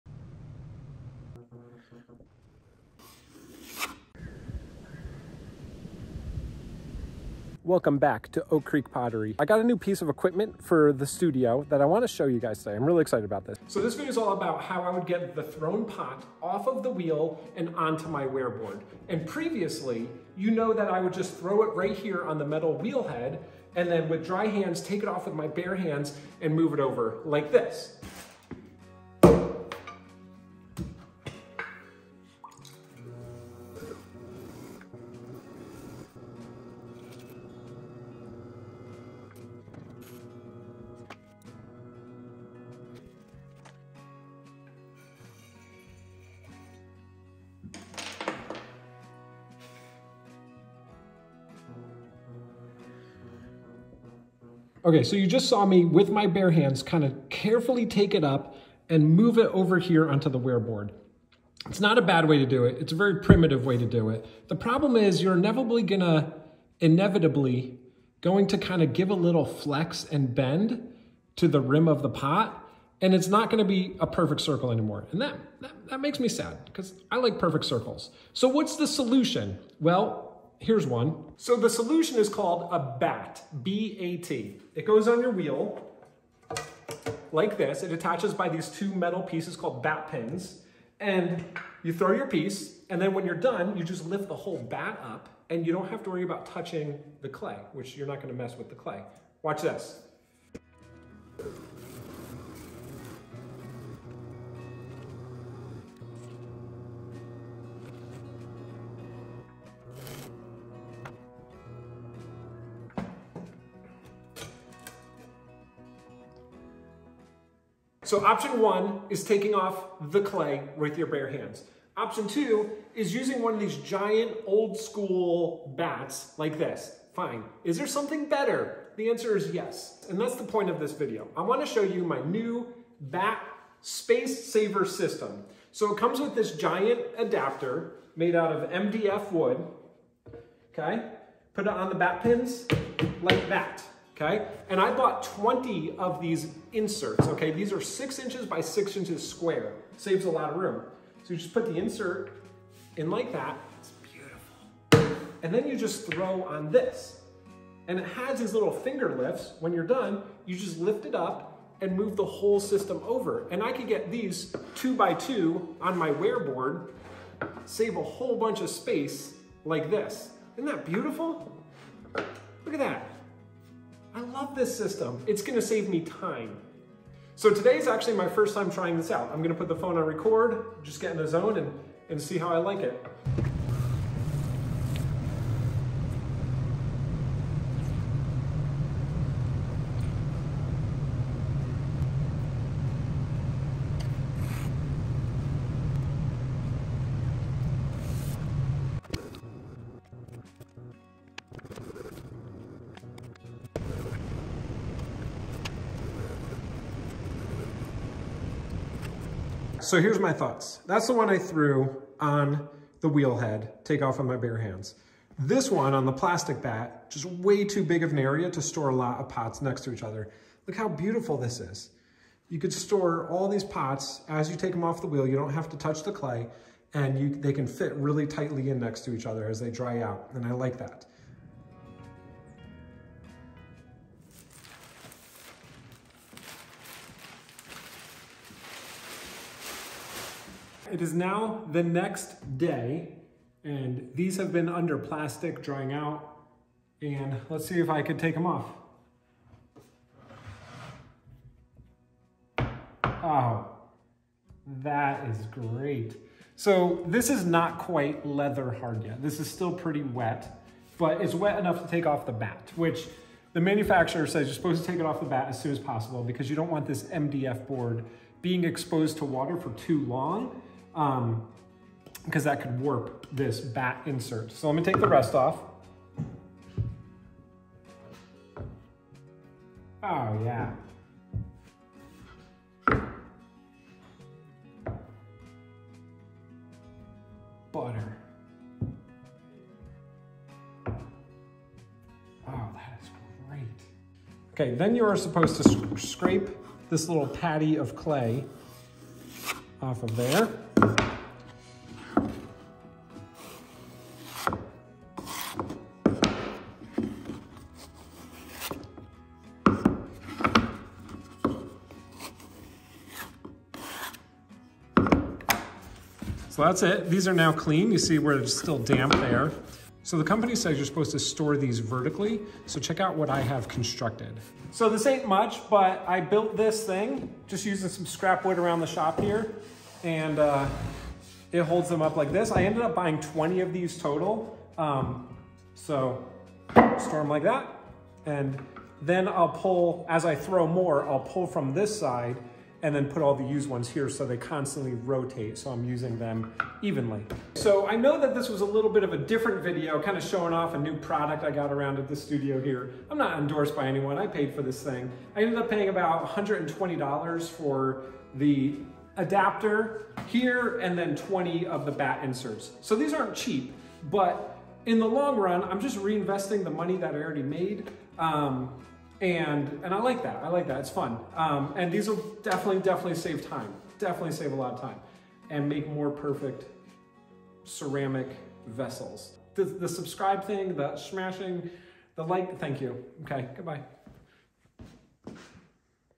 A creative solution to making pottery... bat system review
Update: This is the same video I posted two days ago - the music mixx was way too loud in that last video. I fixed it for this one

In this video, I show off my new bat system. I demonstrate how it saves time and space, and I also give it a review